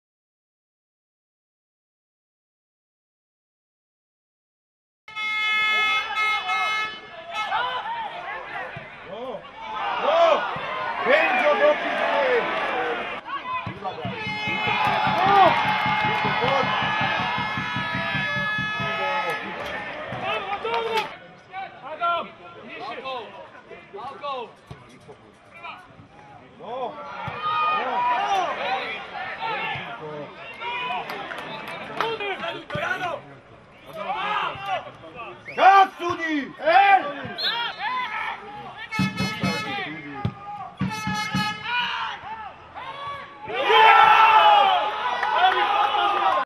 Slovnaft Cup: Malženice - Prešov 0:1
Slovnaft Cup – 4. kolo:
Malženice – Prešov 0:1 (0:0)
Gól: 90.+ Rolinc
Rozhodoval: Valášek
Diváci: 650
MALŽENICE: Močko – Lulovič, Chynoradský, Vdovjak, Hlúch – Rehák, Hutár (70. Braniša), Barborík, R. T. Balážik (60. Pikulík), P. Ľ. Balážik - Benkovský. Tréner: V. Glonek
PREŠOV: Penksa – Petko, Jacko, Dzurík, Haladej (80. Bujňák) - Keresteš (65. Rolinc), Micherda (54. Streňo) - Leško, Katona, Ashiru - Tubonemi. Tréner: S. Varga